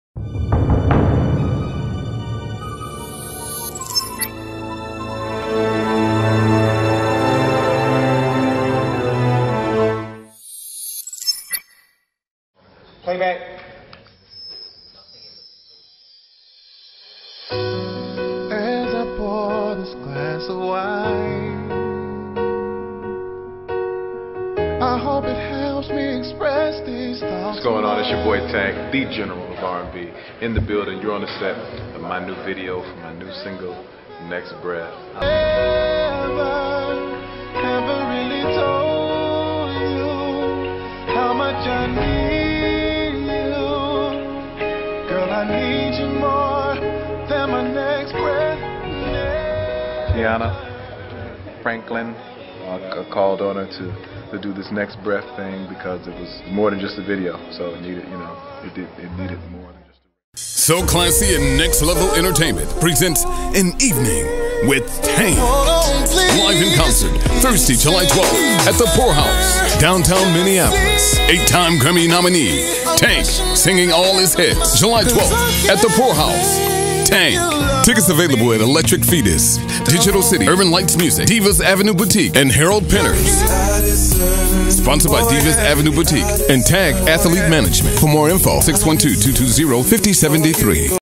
TANK LIVE in Concert at The Pourhouse
SO CLASSY & NEXLEVEL ENTERTAINMENT PRESENTS... TANK

WITH THE #1 ALBUM ON THE BILLBOARD AC AND R&B CHARTS, TANK WILL BE LIVE IN CONCERT AT THE POURHOUSE, DOWNTOWN MINNEAPOLIS'S NEWEST AND MOST EXCLUSIVE MUSIC VENUE.

THURSDAY JULY 12TH 8PM GET READY FOR AN EVENING WITH THE GENERAL OF R&B AS HE TAKES YOU THROUGH HIS PLAYLIST OF #1 HITS.